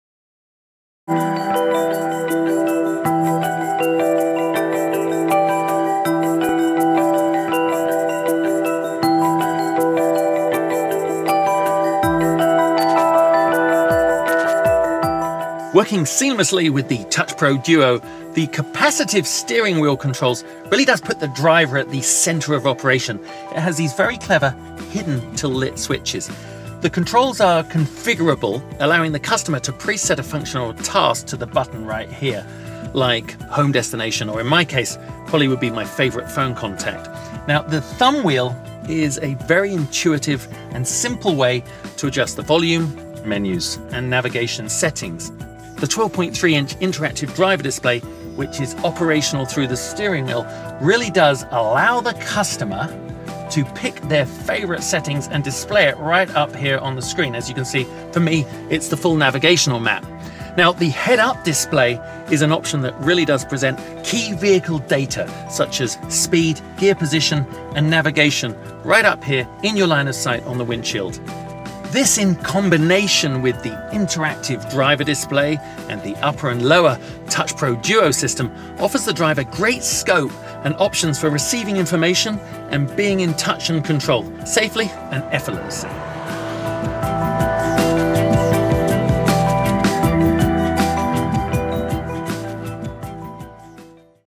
Range Rover Velar | Interior Technology | Land Rover USA
The Range Rover Velar capacity of steering wheel controls puts the driver at the center of operation. The controls are configurable, while the interactive driver display allows the driver to pick their favorite settings shown.